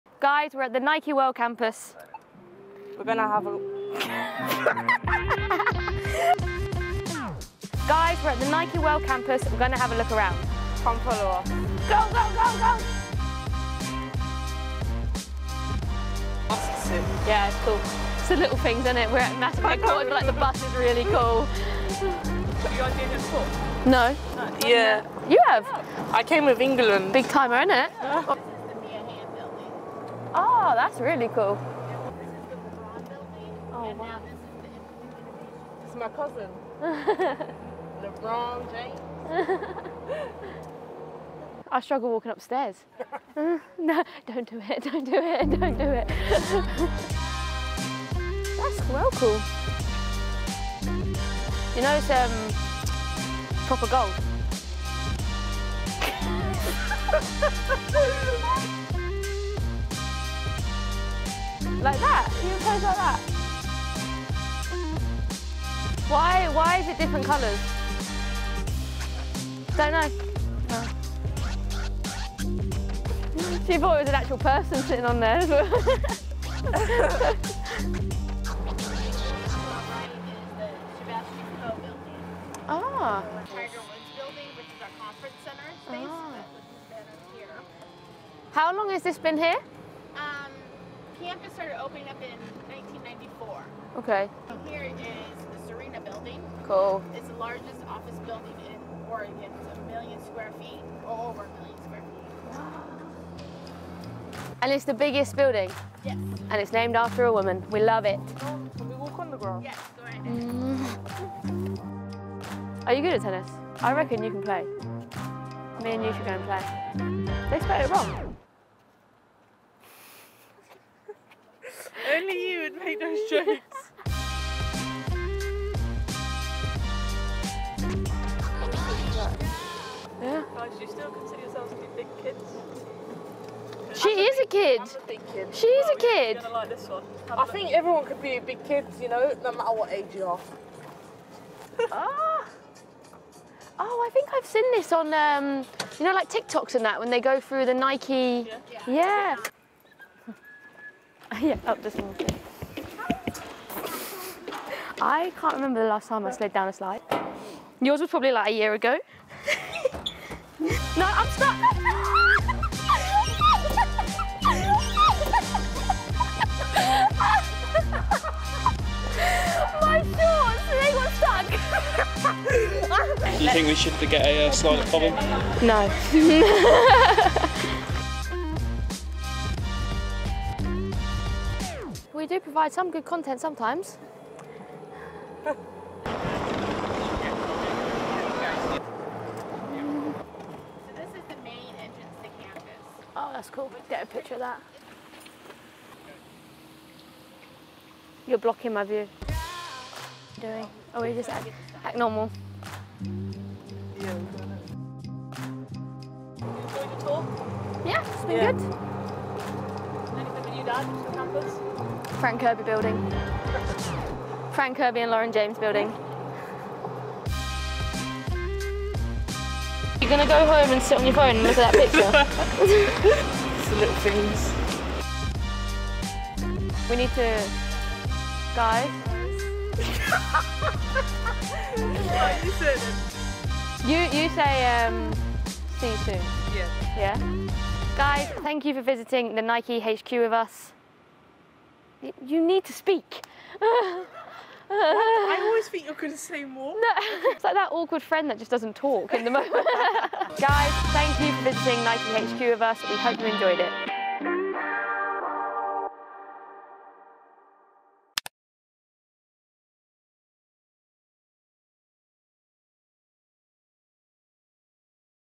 Fran Kirby & Lauren James Take a Tour Around Nike World HQ
Follow Fran Kirby and Lauren James around Nike World HQ.